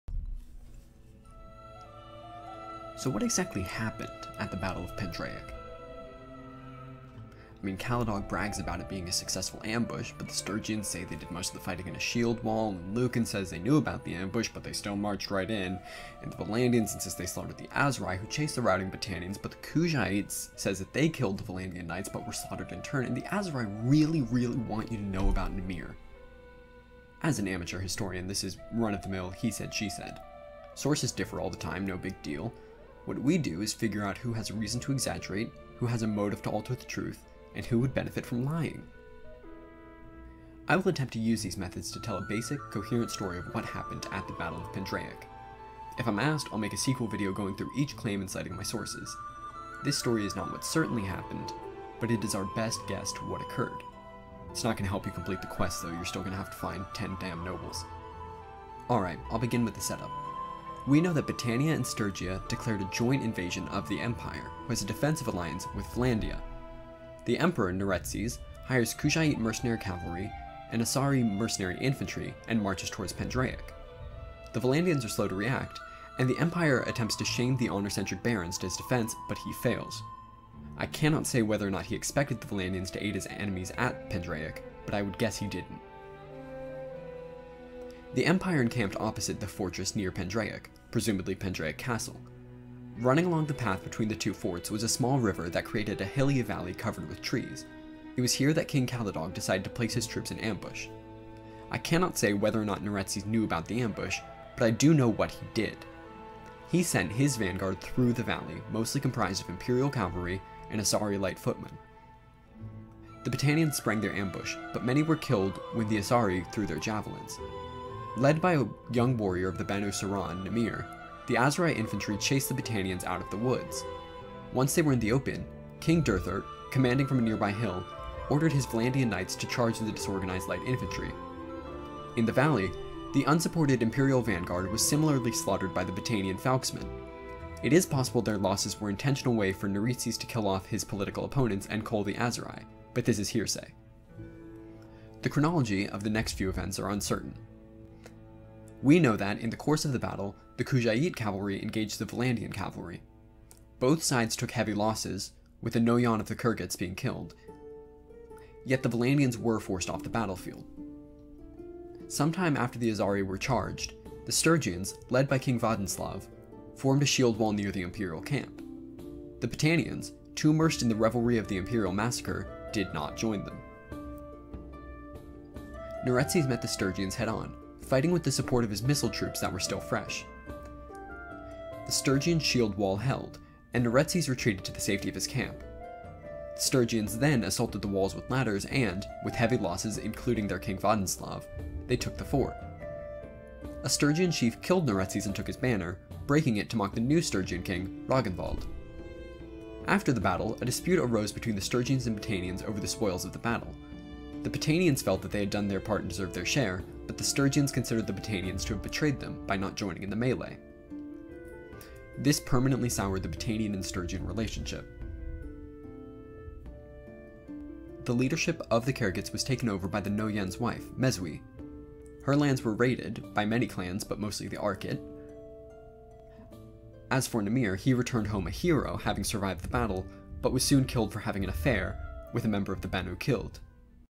So what exactly happened at the Battle of Pendraic?
In which I  attempt to piece together what happened to poor old Neretzes so Caladog can stop bragging about it.

And yes, Derthert is a hill-camper.

What can I say, the developers are good worldbuilders.